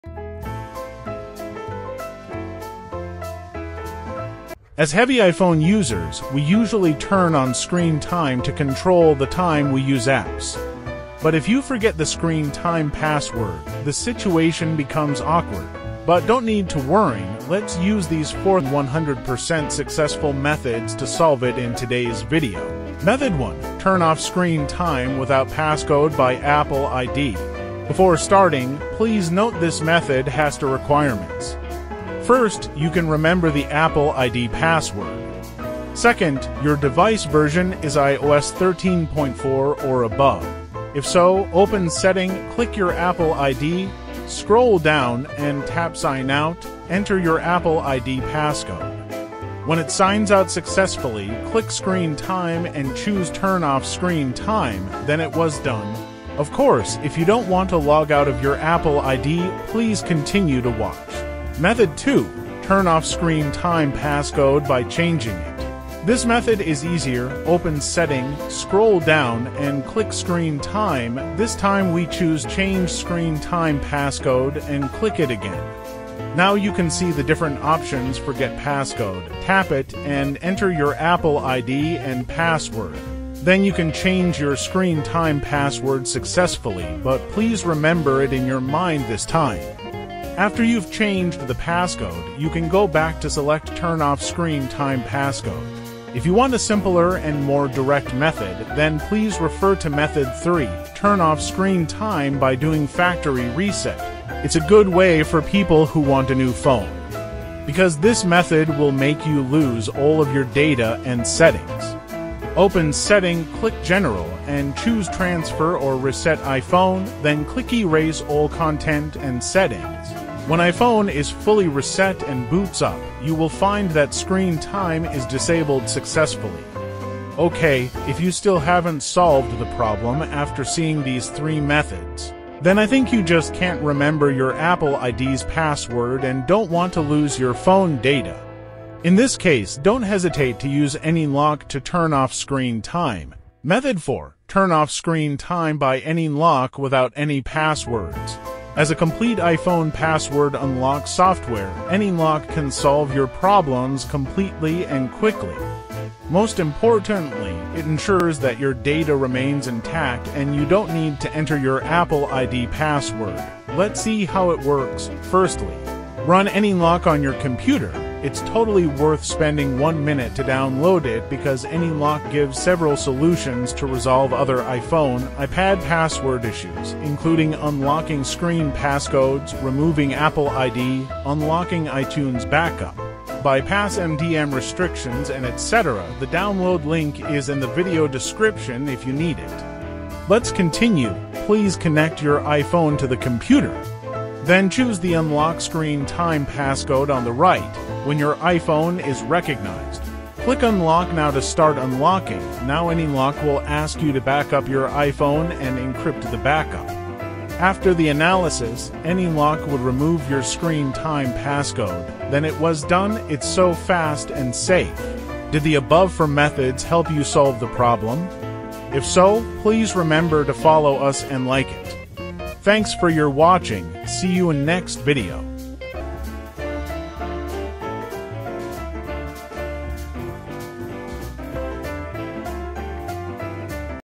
[100% Works] How to Turn off Screen Time on iPhone without Password?
Forget the screen time password to unlock iPhone? Here are four 100% successful methods to turn off screen time on iPhone without password!

As heavy iPhone users, we usually turn on screen time to control the time we use apps. But if you forget the screen time password, the situation becomes awkward. Don’t need to worry! Keep watching this video and find the way!
00:00 Intro
00:27  Method 1. Turn Off Screen Time without Passcode by Apple ID.
01:05  Method 2. Turn Off Screen Time Passcode by changing it.
01:43  Method 3. Turn off Screen Time by doing Factory Reset.
02:27  Method 4: Turn off Screen Time by AnyUnlock without Any Passwords.